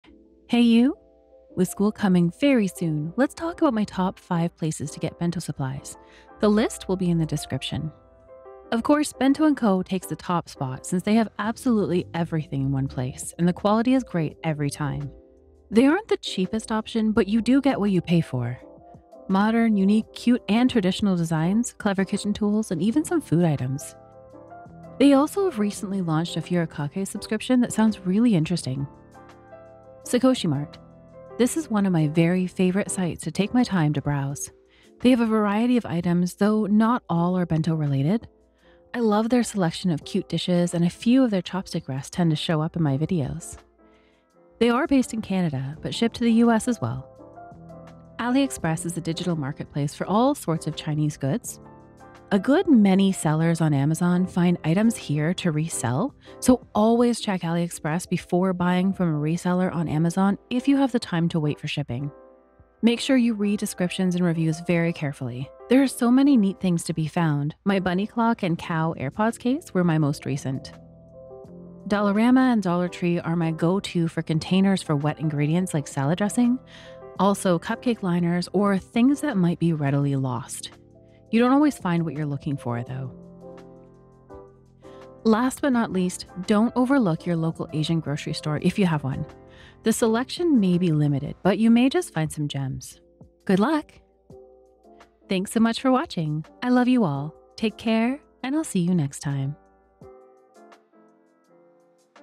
My Top 5 Places to Shop for Bento Supplies!
School is coming quickly,  are you ready?  Here are my favourite places to shop for bento (and other) things.

Bento and co:
Currently have a sale on until September 1st, use code BACKTOSCHOOL